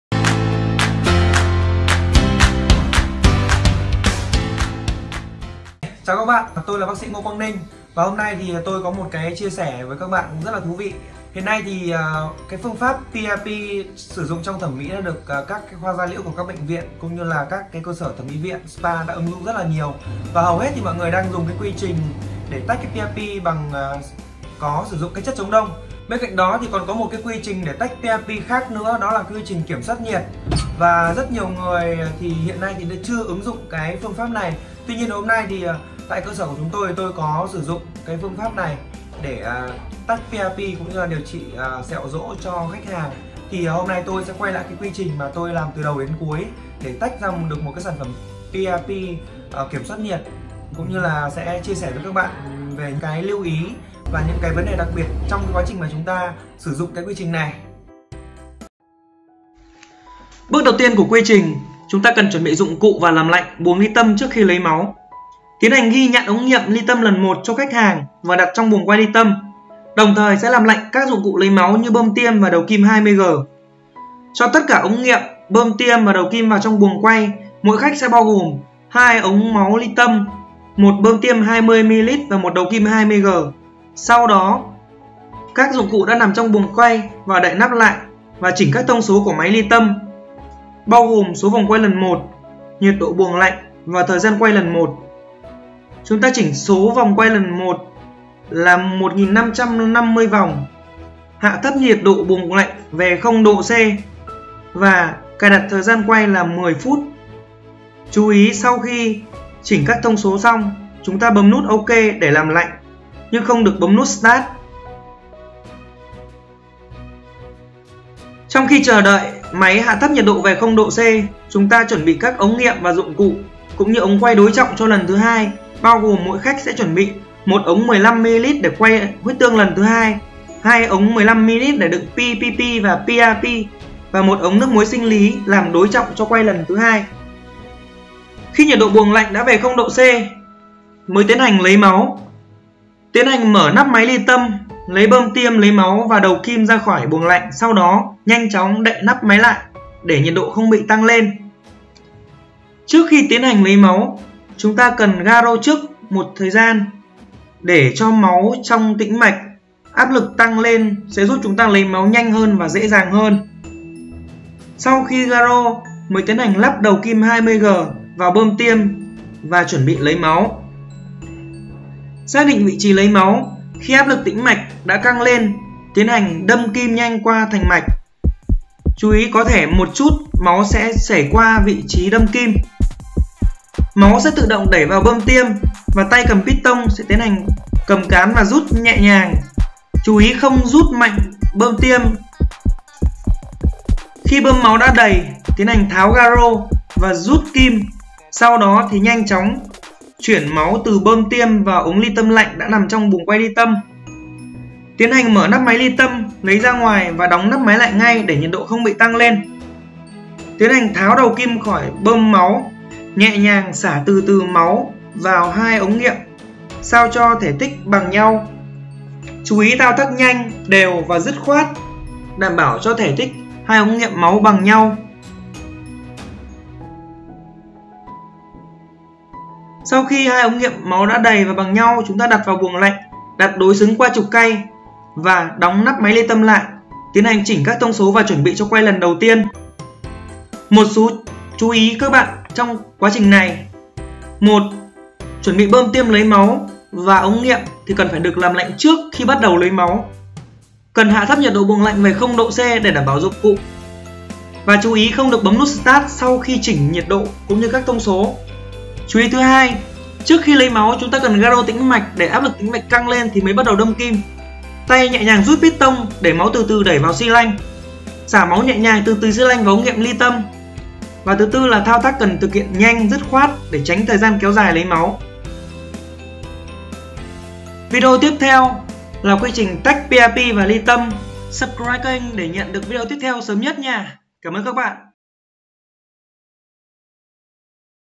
PRP platelet rich plasma| Phương pháp PRP kiểm soát nhiệt không dùng chất chống đông mới nhất 2019
Lấy máu ly tâm là bước đầu tiên cực kỳ quan trọng trong quy trình tách huyết tương giàu tiểu cầu PRP( platelet rich plasma ), đặc biệt quan trọng trong  ly tâm phương pháp PRP không chống đông máu bằng ACD-A.
Huyết tương giàu tiểu cầu(máu tự thân) hiện nay được ứng dụng rất rộng rãi trong y học và thẩm mỹ da. Ngoài quy trình tách PRP thông thường có sử dụng chất chống đông, hôm nay Dr Ninh chia sẻ với các bạn quy trình tách PRP kiểm soát nhiệt độ quá trình ly tâm ở 4 độ C, giữ cho tiểu cầu không bị vỡ. Quy trình tách PRP này đòi hỏi người làm phải thành thạo từ quy trình chuẩn bị dụng cụ, lấy máu, đến tách hút PRP với kỹ thuật khó hơn. Vì vậy nếu bạn nào muốn học quy trình này thì hày xem kỹ video này và video phần 2, vì trong video Dr Ninh đã ghi lại khá chi tiết cách thực hiện và những lưu ý.

Tham vẫn chuyên gia:
HÃY CHIA SẺ GIÚP BÁC SỸ NHÉ! THANK'S VERY MUCH!